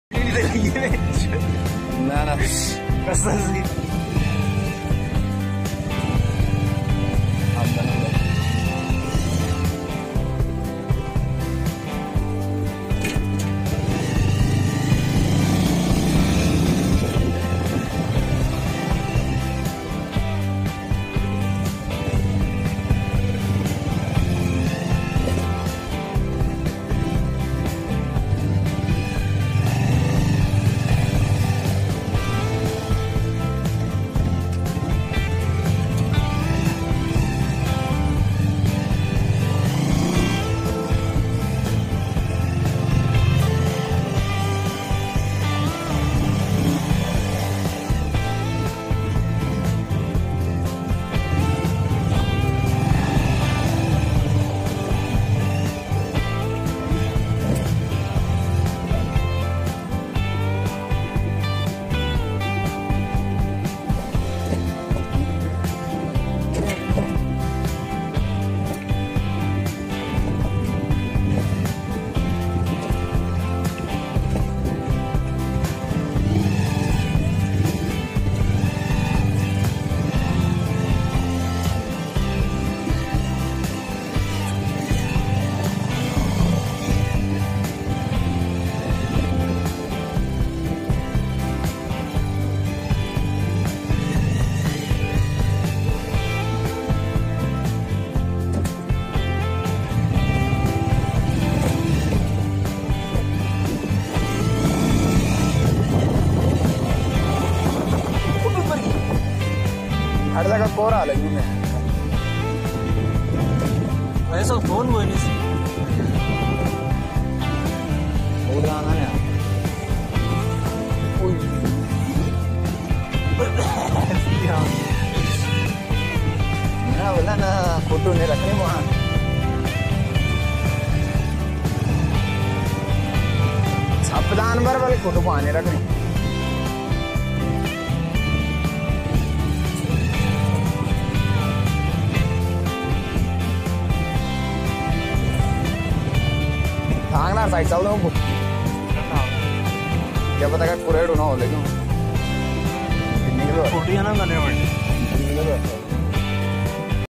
Off Road Night Out Kanatal Kaudia Forest
Off Road With Friend Kaudia Forest Kanatal.
 This Session Awesome View of Forest.